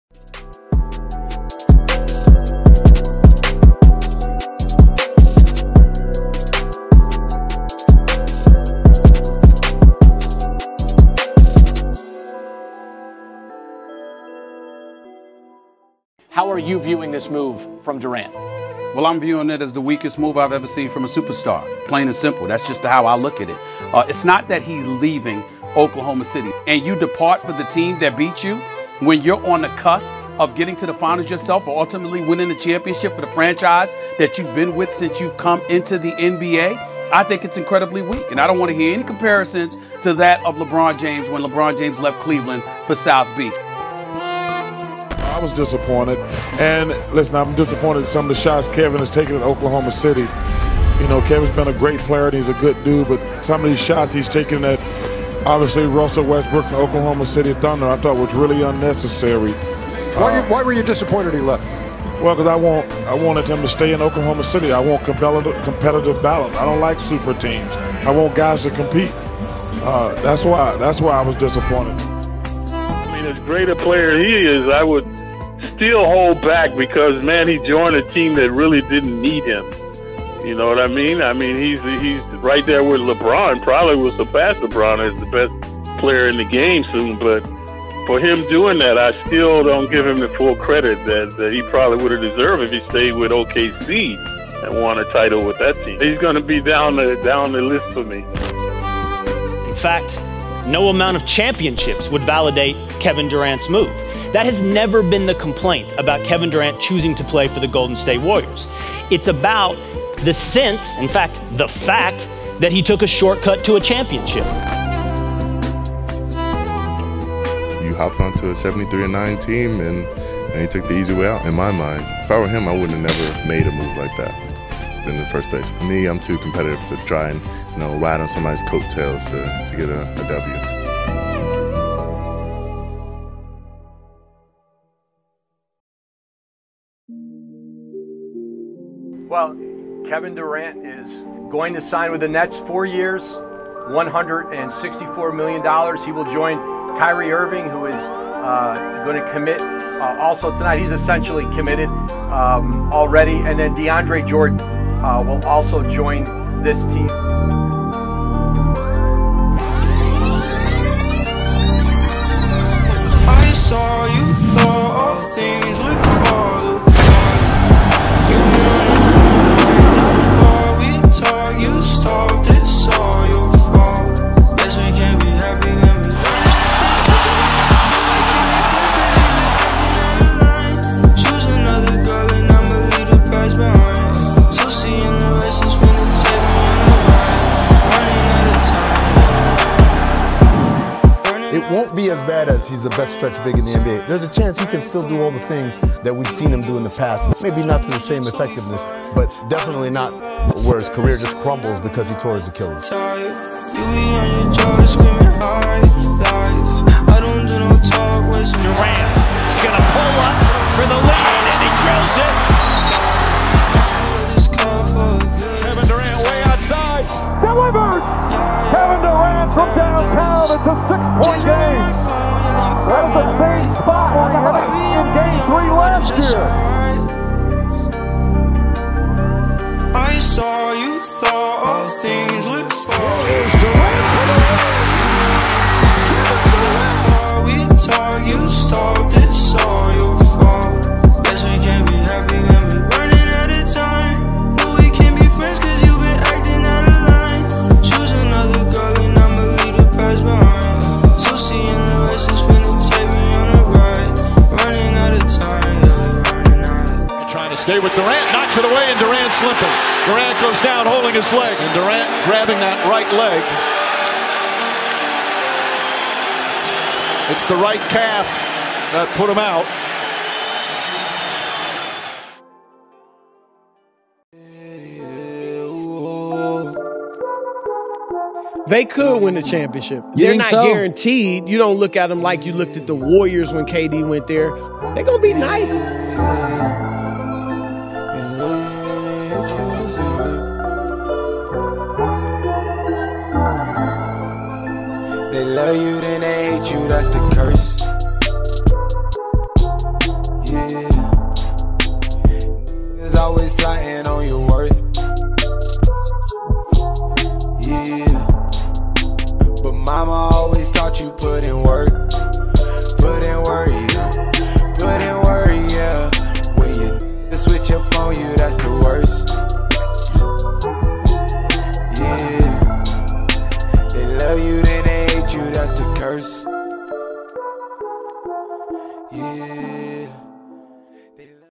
Kevin Durant - Renewed Legacy (NETS HYPE) ᴴᴰ
Kevin Durant Nets Pump Up/Highlights/Promo/Trailer/Motivational Mix/Tribute

Watermark is added because it is a sense of pride in my work. It lets others now I put together the elements of this video although the clips are from sports networks.